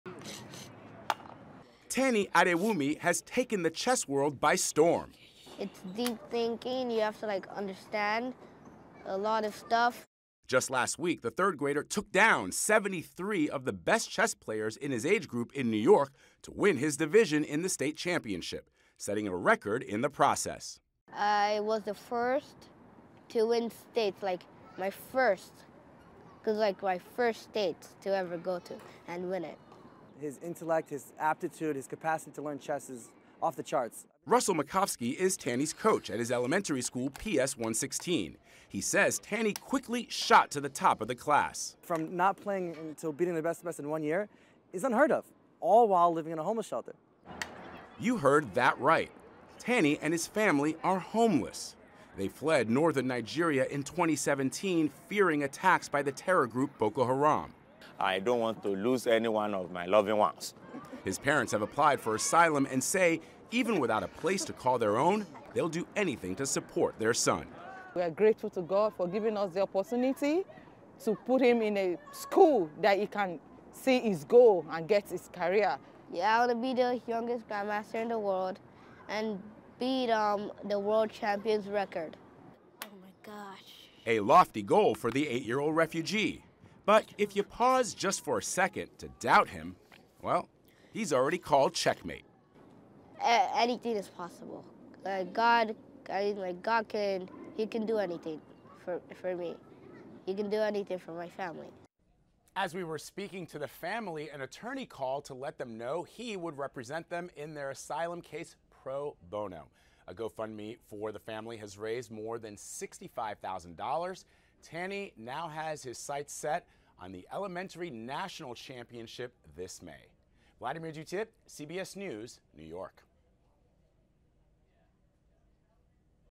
8-year-old chess champ making right moves | 10News WTSP
His story has raised enough donations to move his refugee family out of a shelter and into an apartment.